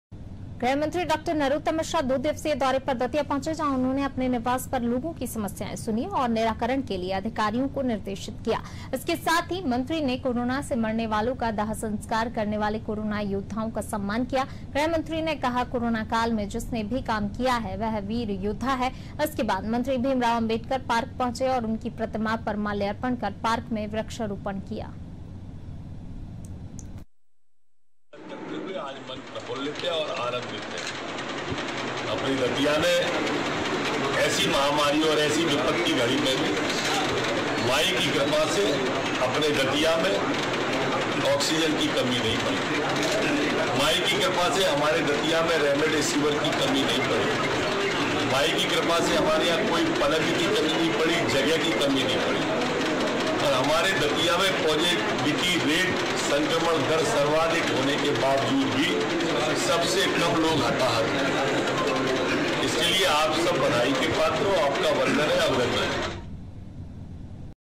दतिया : गृह जिले के दौरे पर गृहमंत्री |Top News today Hindi | Breaking News Hindi | Mp news
ग्रहमंत्री डॉ नरोत्तम मिश्रा दो दिवसीय दौरे पर दतिया पहुंचे ... जहां उन्होने अपने निवास पर लोगों की समस्याएं सुनीं ... और निराकरण के लिए अधिकारियों को निर्देशित किया इसके साथ ही मंत्री ने कोरोना से मरने वालों का दाहसंस्कार करने वाले कोरोना योद्धाओं का सम्मान किया ... गृह मंत्री ने कहा कोरोना काल में जिसने भी काम किया वह वीर योद्धा है ... इसके बाद मंत्री भीमराव अंबेडकर पार्क पहुंचे और उनकी प्रतिमा पर माल्यार्पण कर पार्क में वृक्षारोपण किया ...